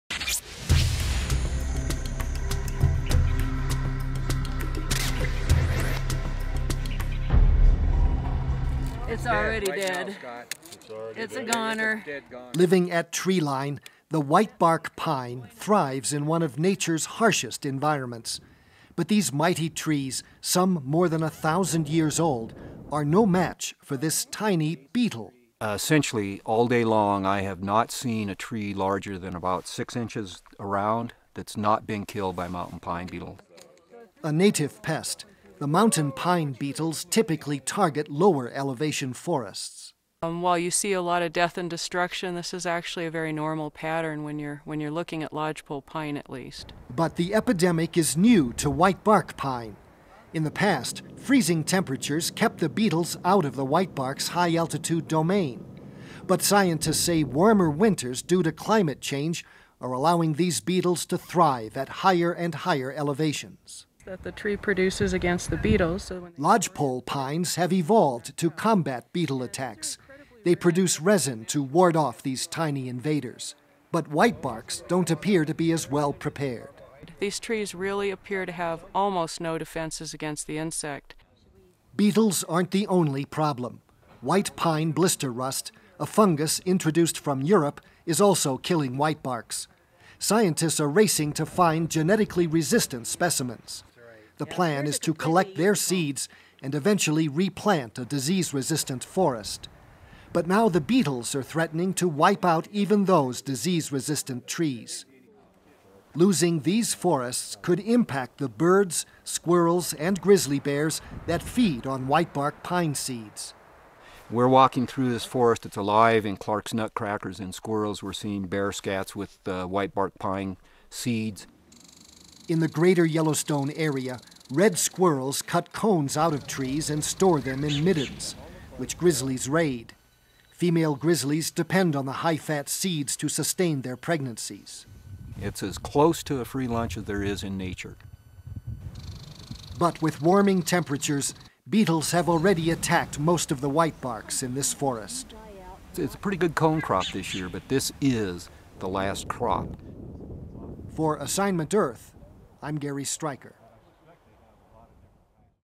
Whitebark Pine Disaster
In the Northern Rockies, beetle infestation and invasive fungus are destroying ancient whitebark pine forests, with serious impacts on wildlife.